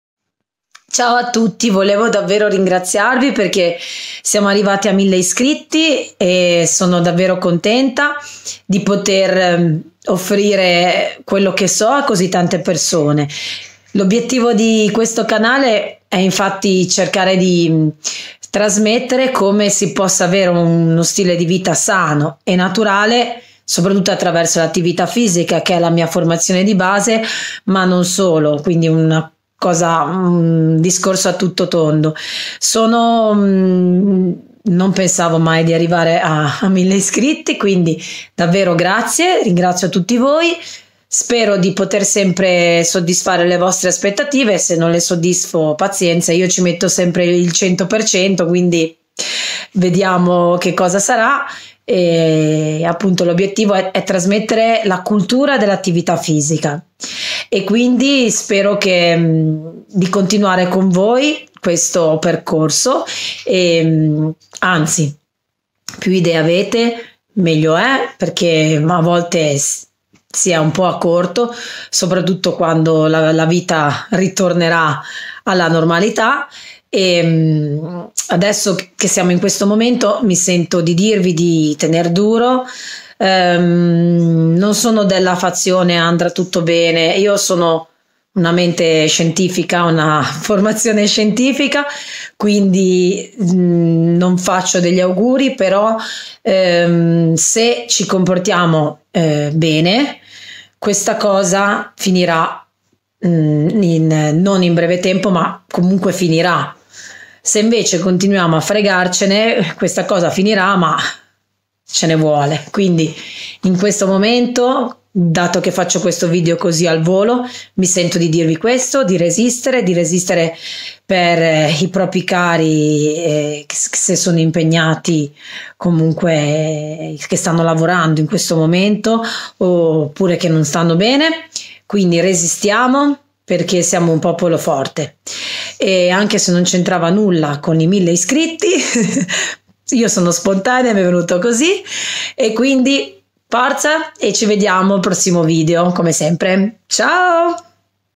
1000 GRAZIE!!!!!
Grazie davvero tanto, 1000 iscritti wow!!!

Spero davvero di aiutarvi a prendervi cura di voi stessi grazie all'attività fisica e di tenervi compagnia!

Un abbraccio dalla Liguria!